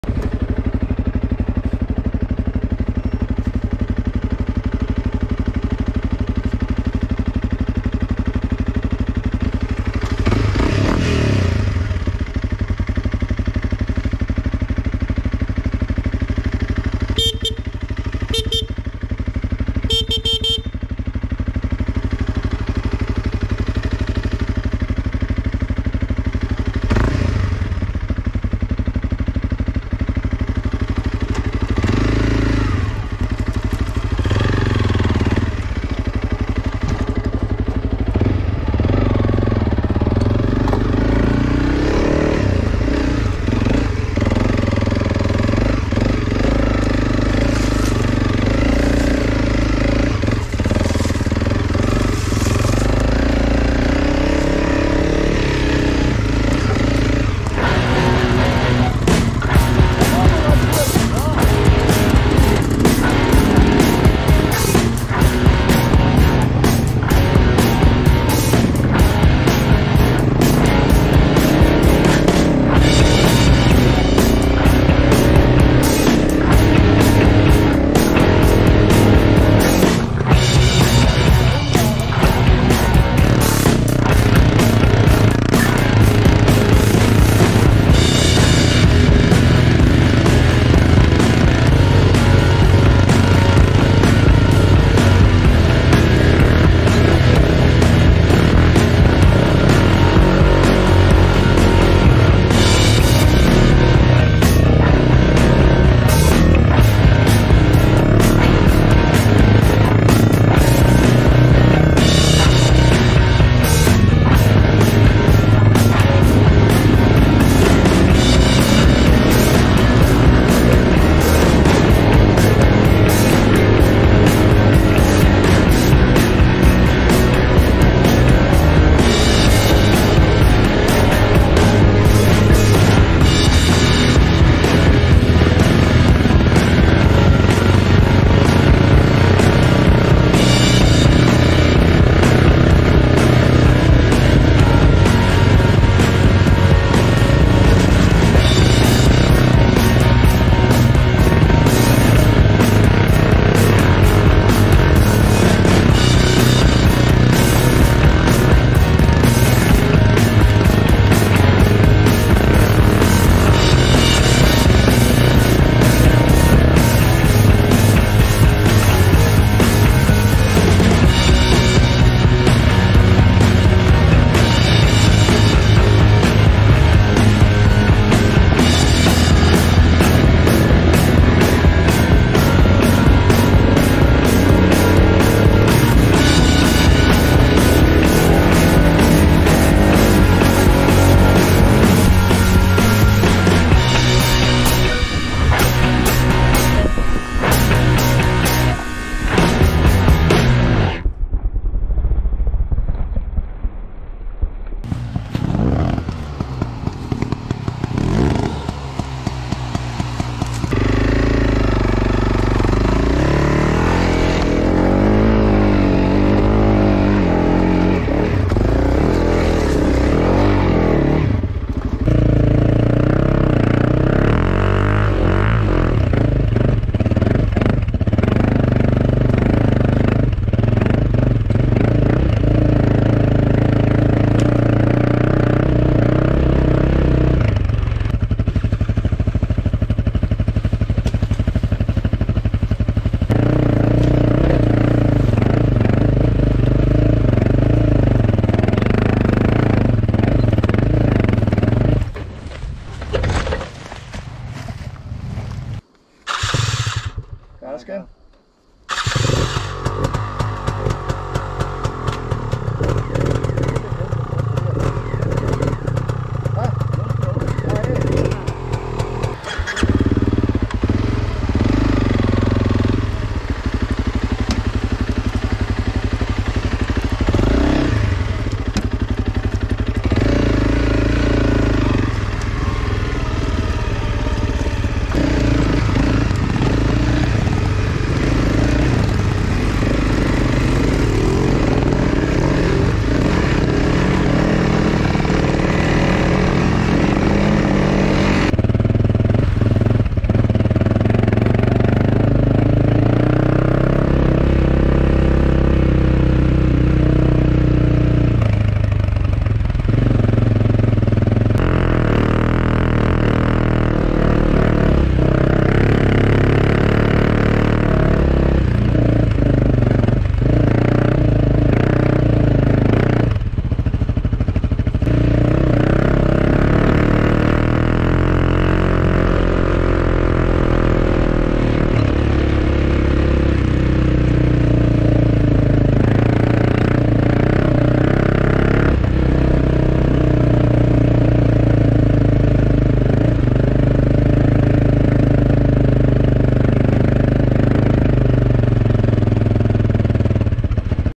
Riding offroad in Bosnia with my CRF300 Rally.
Till now i have been fallen a lot. I crashed on rocks, on roots, on grass, on leaves, on gravel, on mud and so on. It seems, the CRF is taking it easy. One mirror and the first set of handguard are the only damages i have to report. The bike is doing a good job.